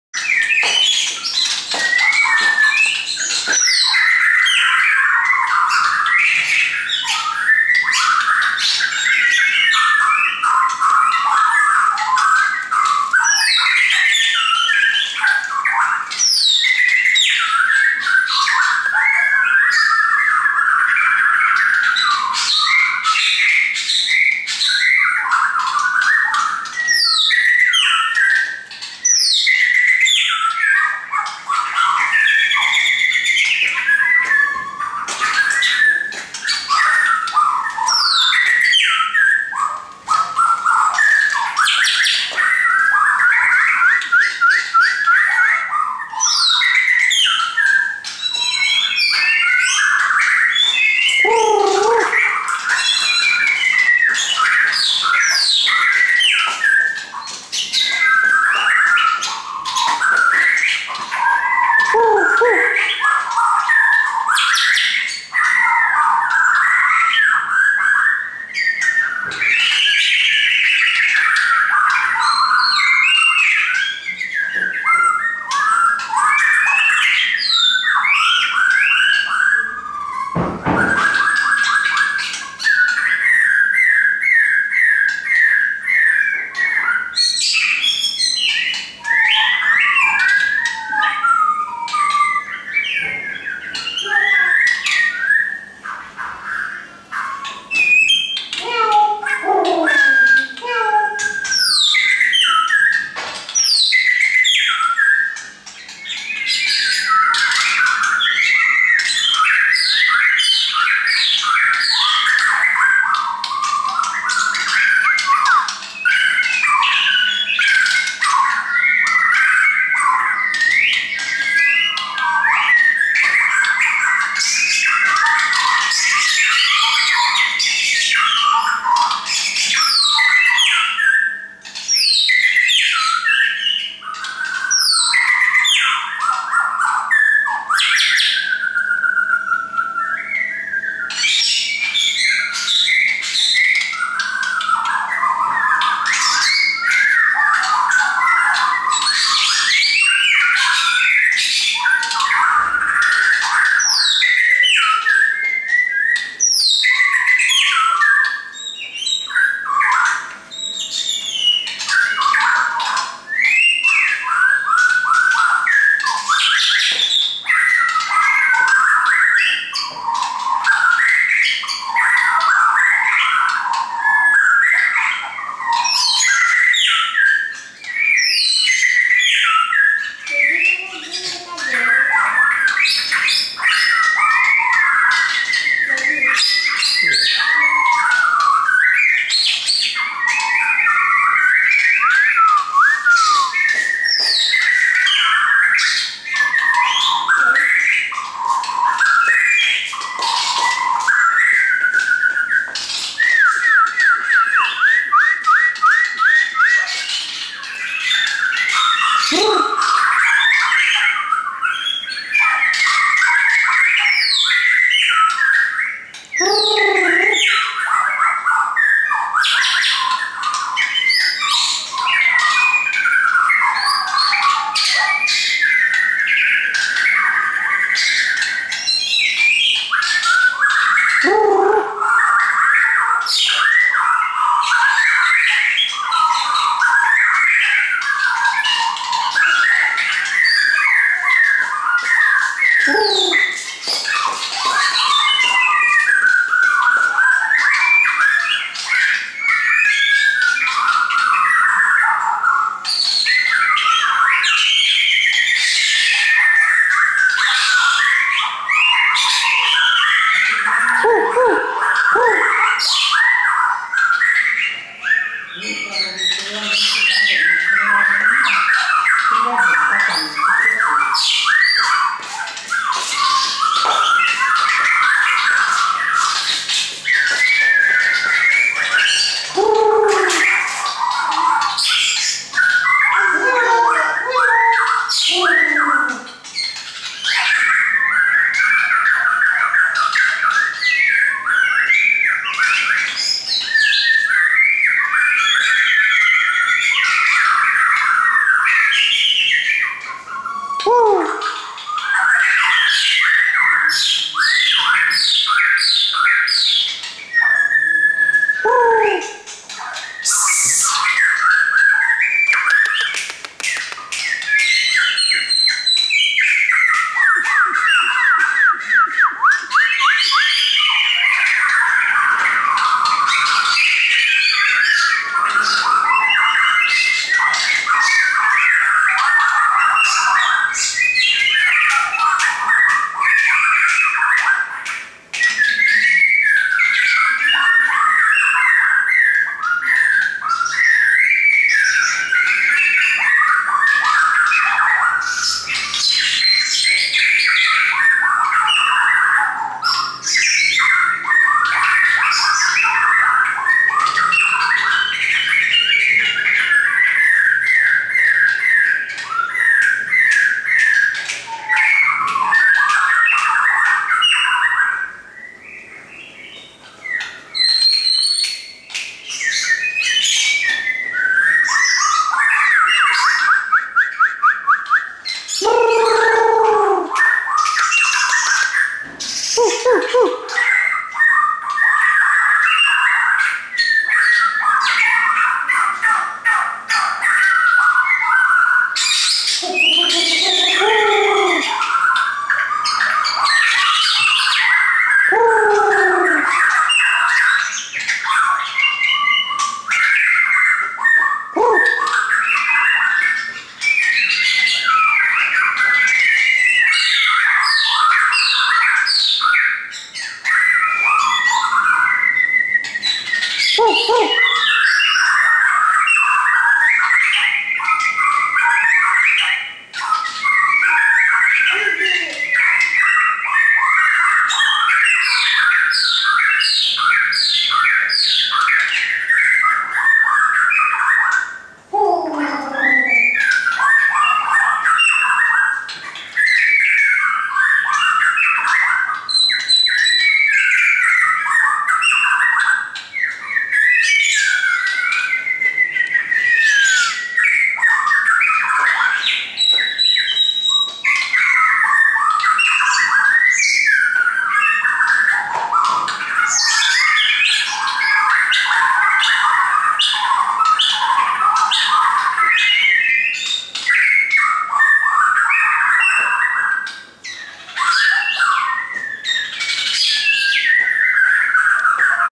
WCS+WRS
A friend brought his shamas for chai. Those shamas belong to him.